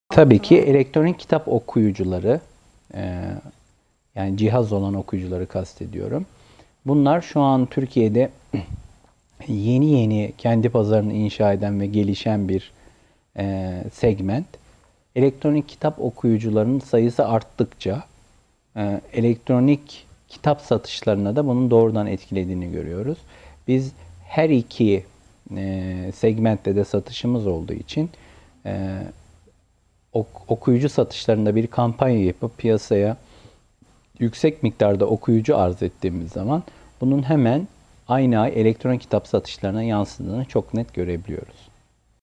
E-kitap ve Dijital İçerik Satışları Arasındaki Orantı [Mehmet Ali Çalışkan]
Sn. Mehmet Ali Çalışkan İle 10 Soruda E-Kitap:

E-KİTAP VE DİJİTAL İÇERİK SATIŞLARI ARASINDAKİ ORANTI